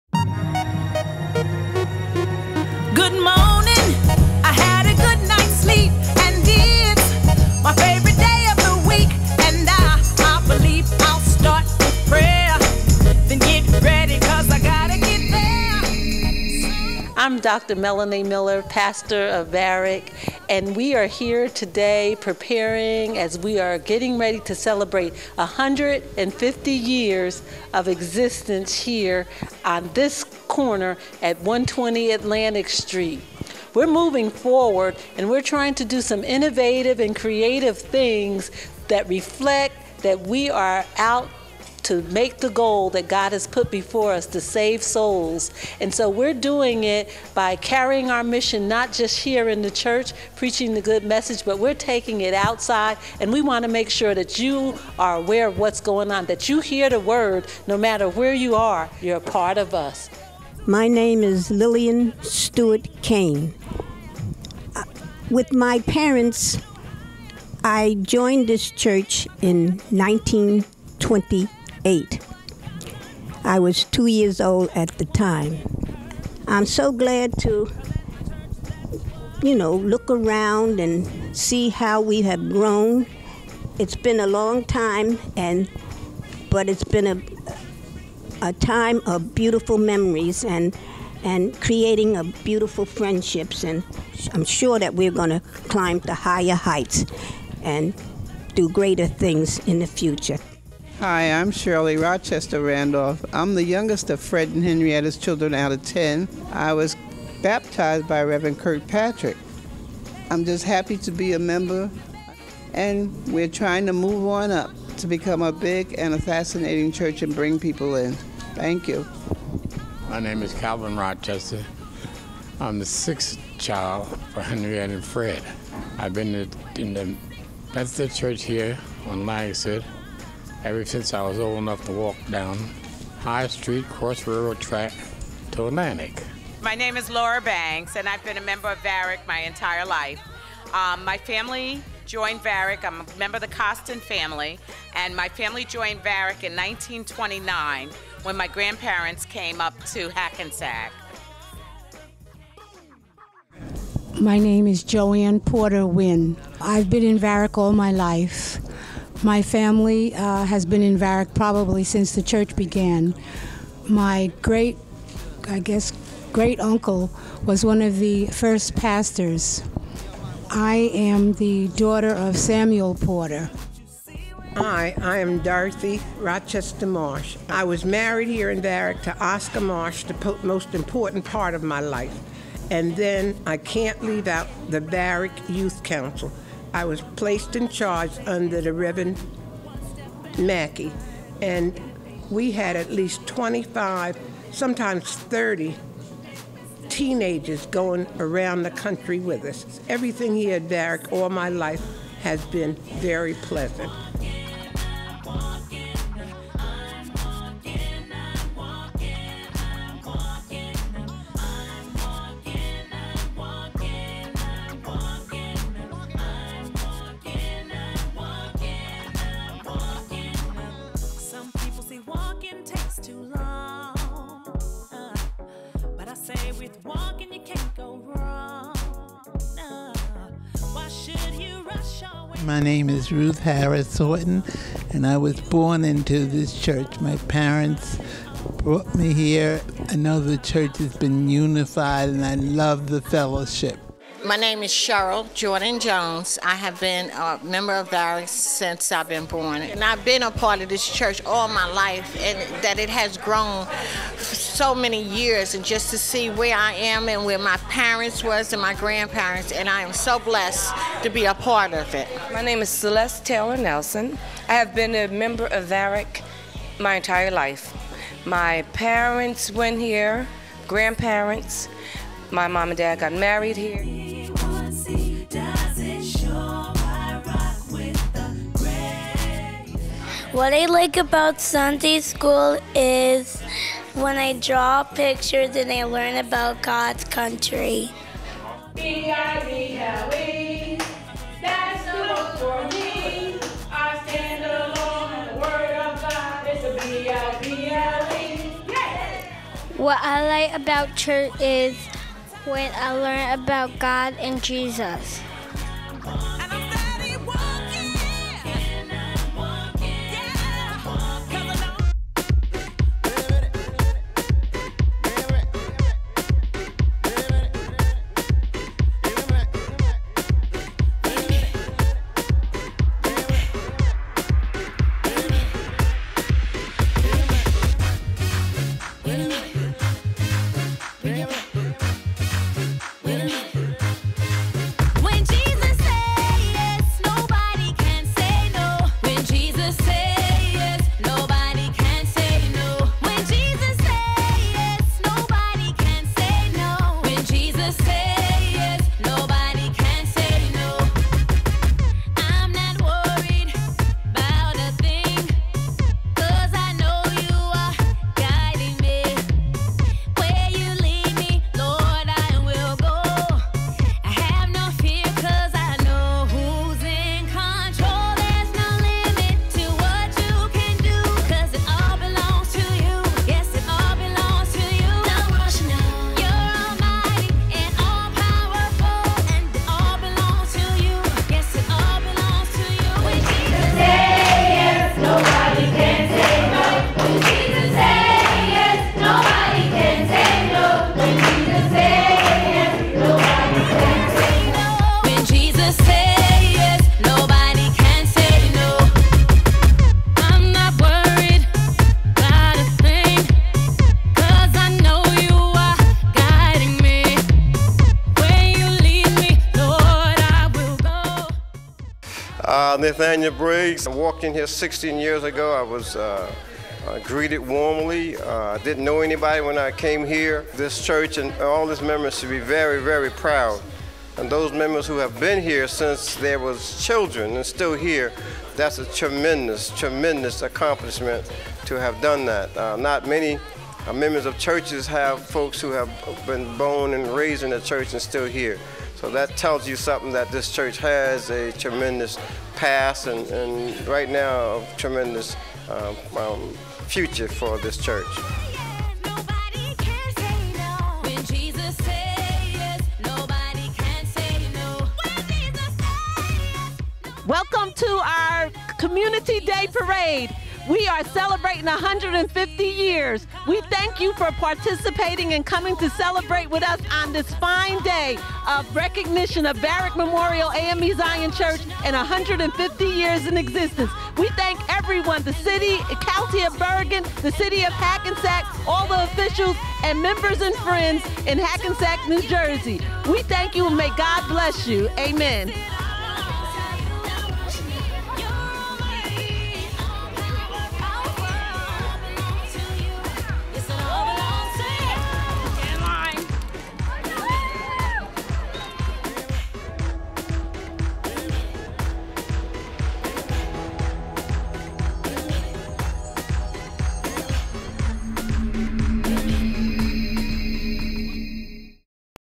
Varick Memorial AMEZ 150th Anniversary
We take a look back at a journey by faith, up to all the things going on at Varick during 2014.  Memories from elders who were born and raised at Varick, including highlights of the anniversary parade and more.